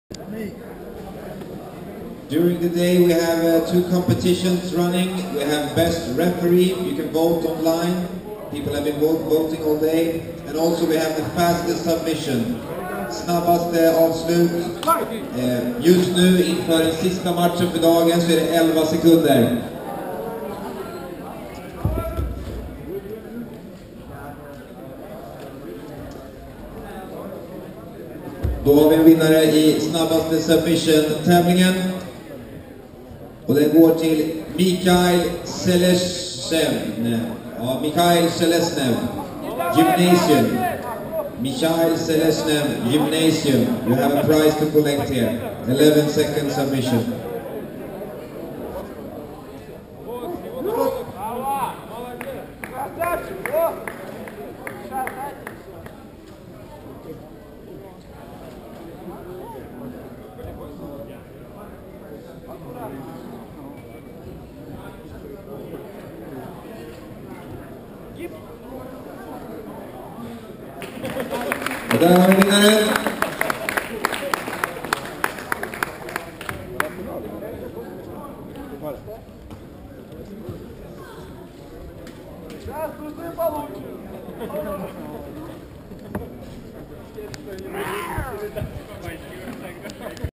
Jungle BJJ - Nordic BJJ Open - Yakuza (06)
Jungle BJJ member Eldar "Yakuza" on his way to another gold in Stockholm, Sweden, on March 9th, 2014.